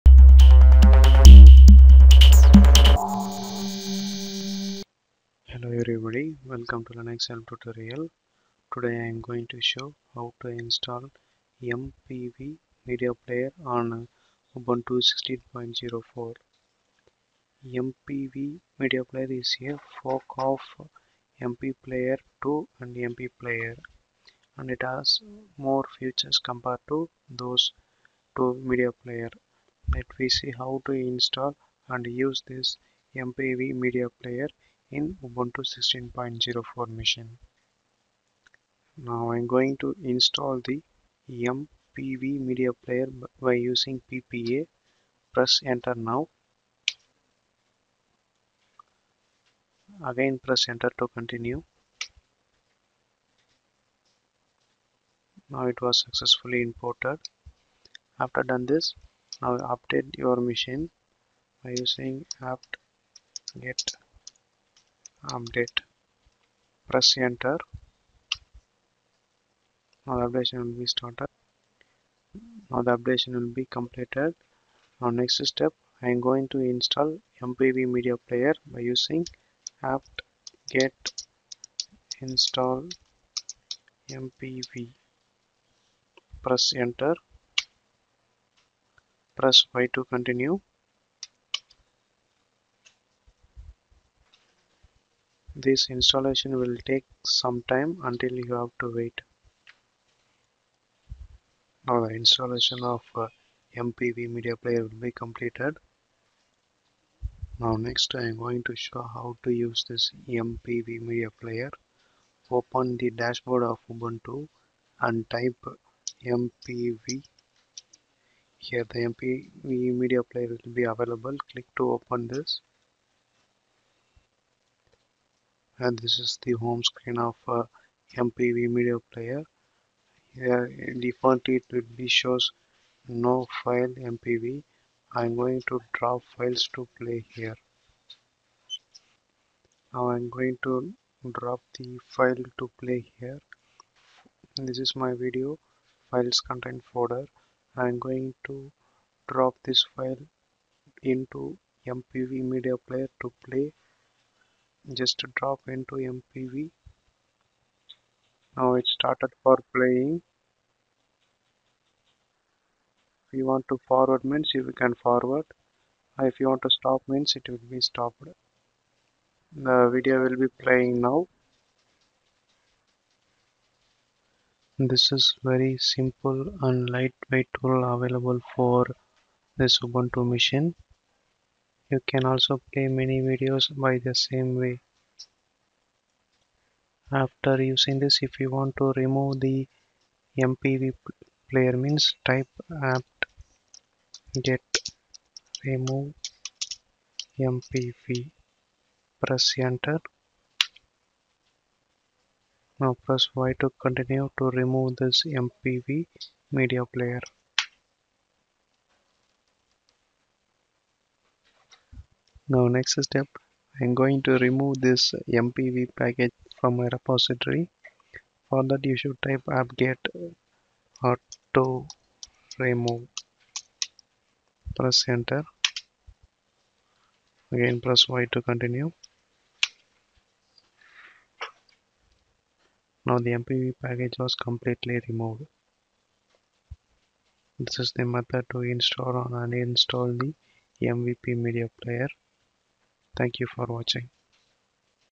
How to install MPV Media Player in Ubuntu
This video explains how to install MPV media player in Ubuntu 16.04. It is a part of mplayer2 and MPlayer.